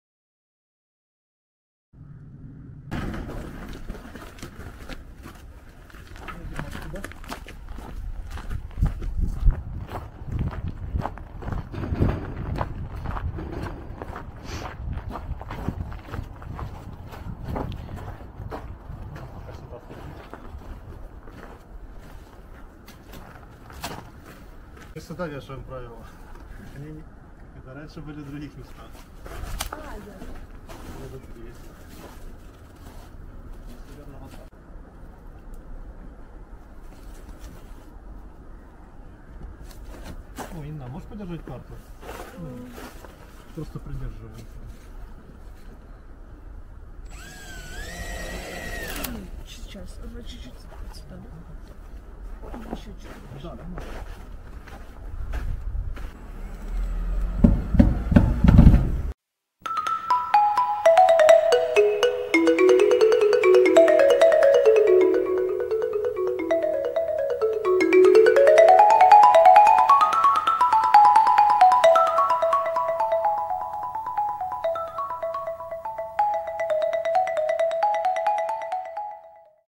Розміщуємо правила Котомістечка
Олег розміщує на паркані правила Котомістечка в Києві. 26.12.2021.
В кадрі: Олег Андрос, співзасновник ГО "Природа над усе".
Відео: Яніна.

Стань нашим патроном!